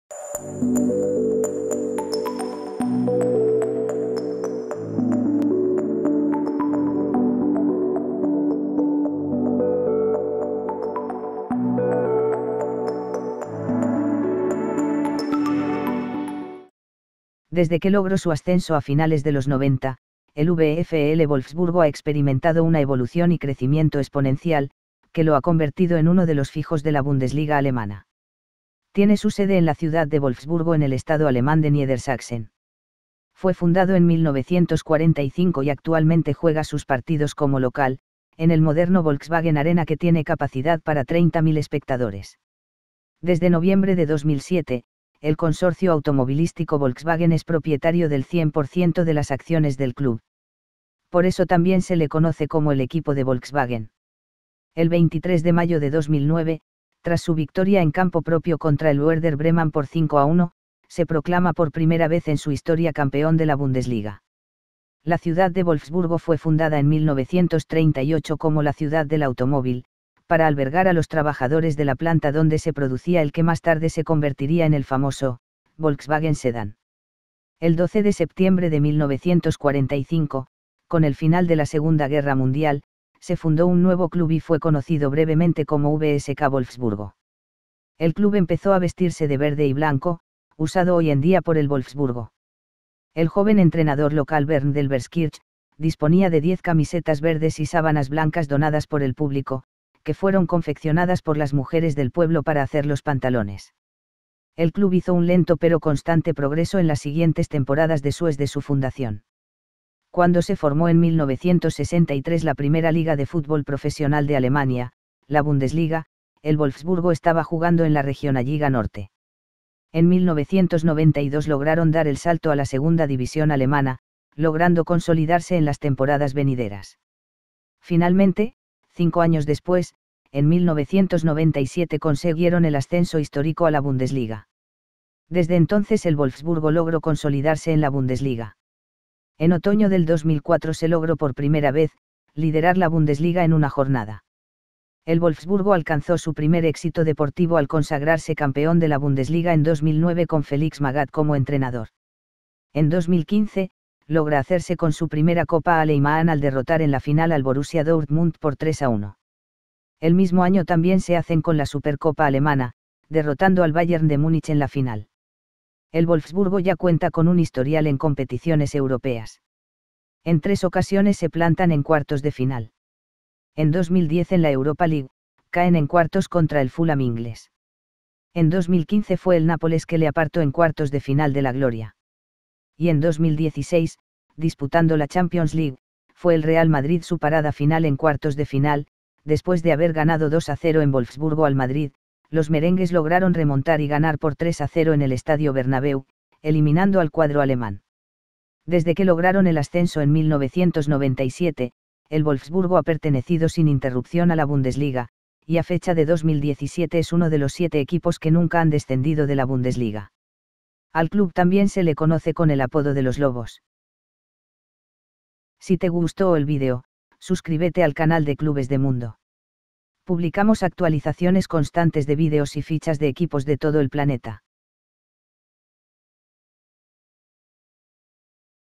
VFL WOLFSBURG - Campeón Bundesliga 2009 y Copa Alemana 2015 - Clubes del Mundo
El VFL Wolfsburgo es un club que fue fundado después de la II Guerra Mundial  En 1993 logra su Ascenso a la 2.Bundesliga alemana,  y 5 años más tarde en 1997 logra el Ascenso a la Bundesliga. A fecha de 2018, desde entonces ha permanecido siempre en la Bundesliga.
En 2009 logra su gran triunfo al coronarse Campeón de la Bundesliga. En 2015 logra la Copa Alemana (DFB Pokal) y la Supercopa Alemana.
El club pertenece a la empresa de Volkswagen.
El Wolfsburgo también llevan el apodo de "Die Wölfe", los Lobos.